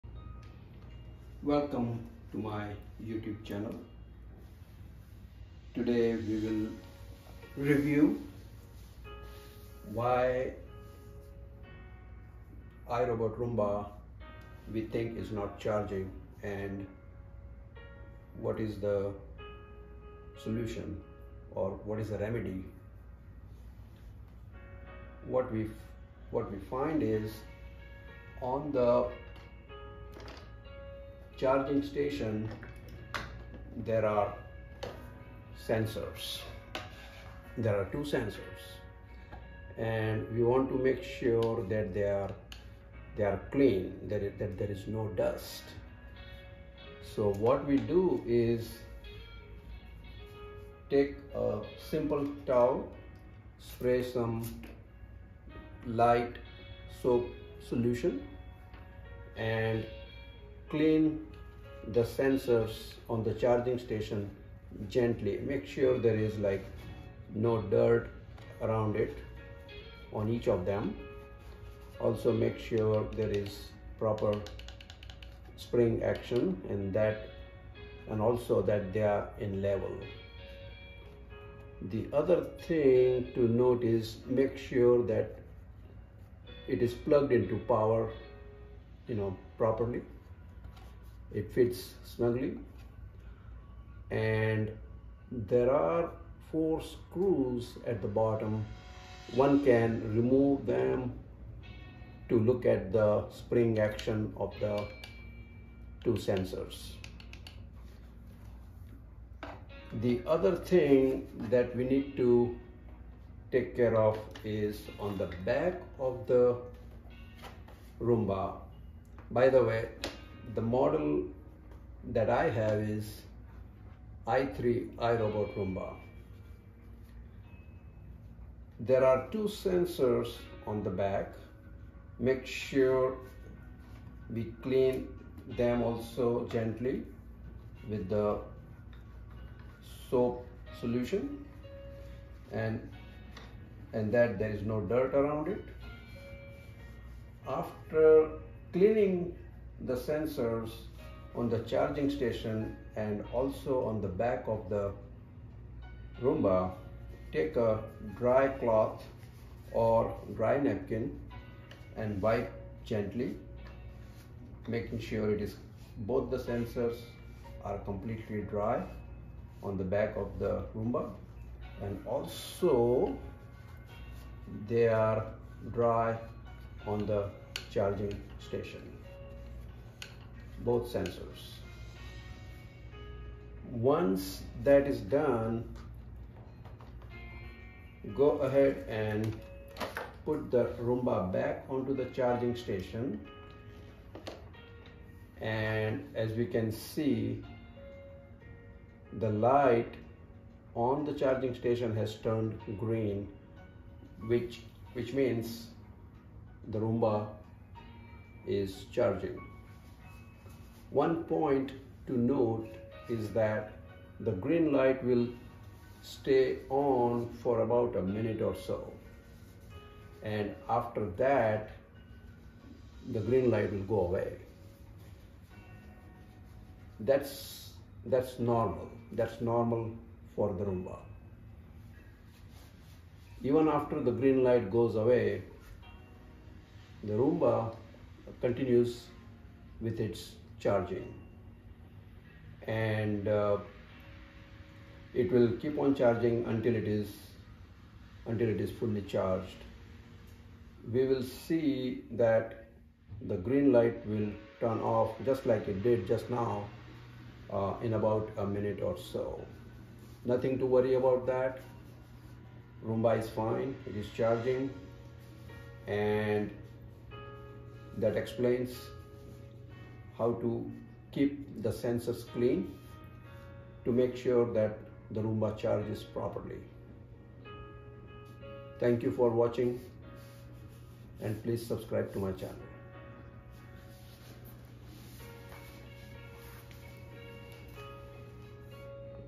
iRobot Roomba i3 - Clean the sensors for charging
This video explains how to clean the sensors of iRobot Roomba i3 so that it's  battery gets charged when Roomba is placed on the charging station.  Also, it explains how to check the sensors on the charging station.